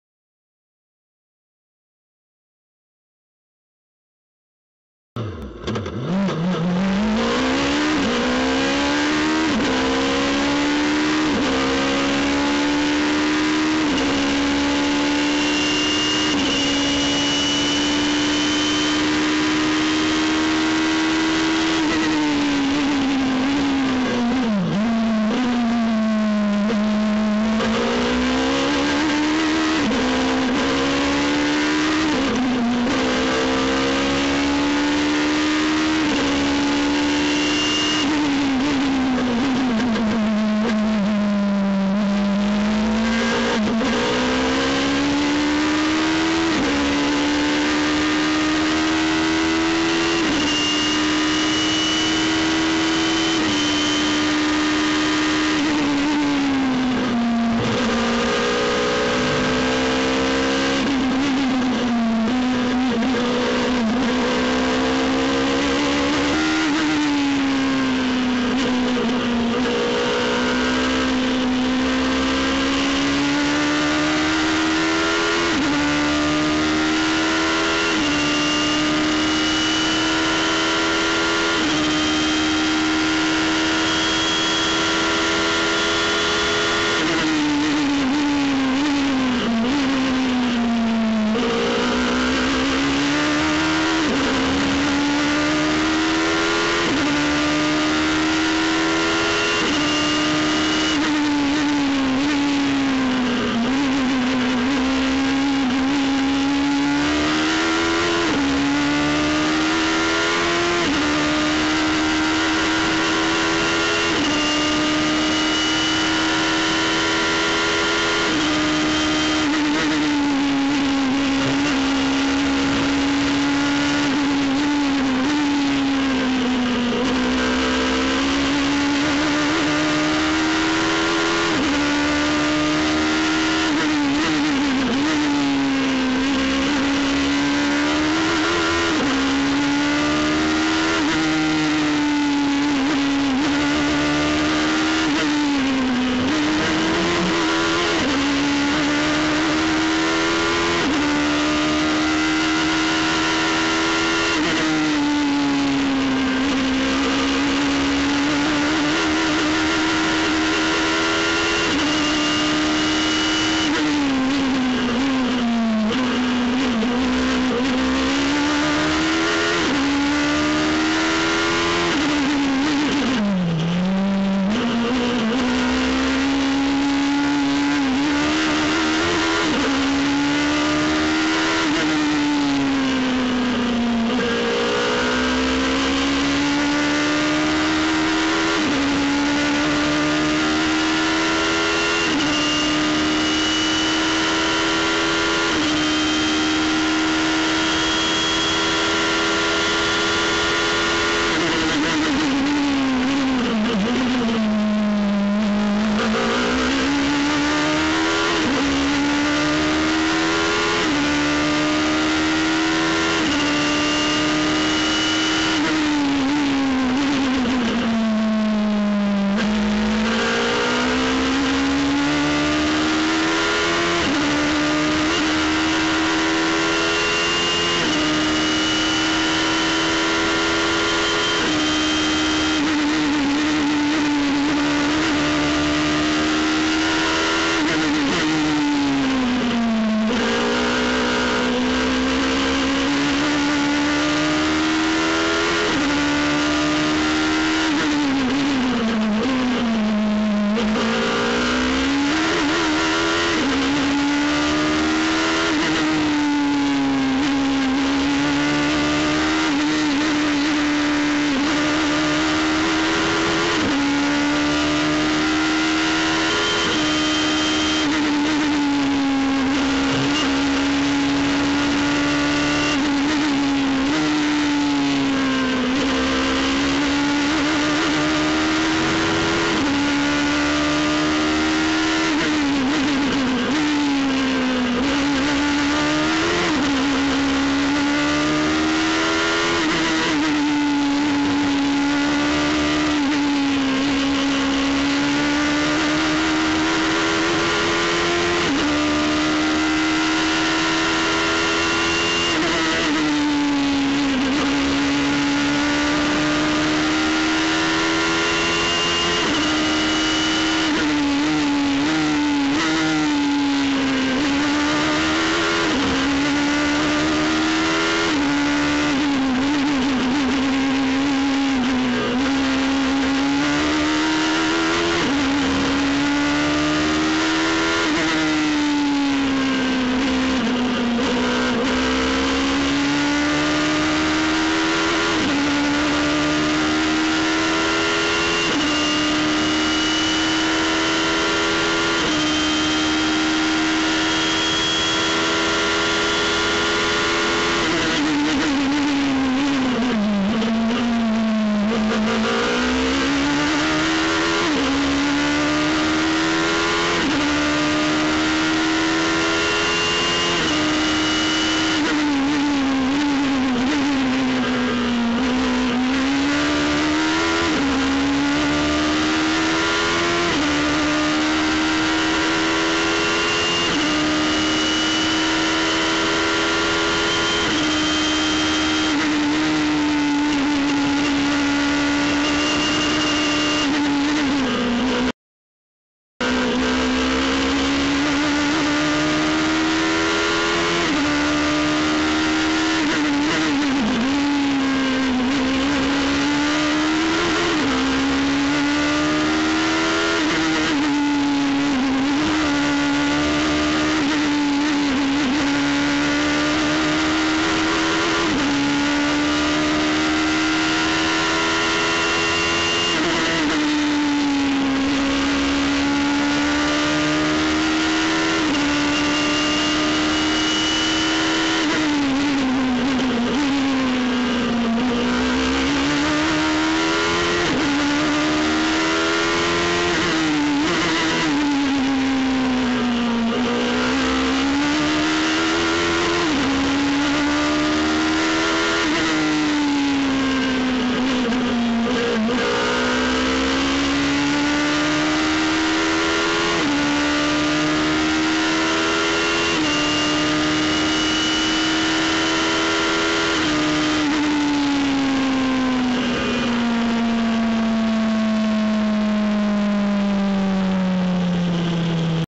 Radical SR3 onboard at Hungaroring Radical Masters with Alex Kapadia
Highlights from the Radical Masters round at Hungaroring, onboard with Alex Kapadia.

Follow Alex Kapadia on twitter @alexkapadia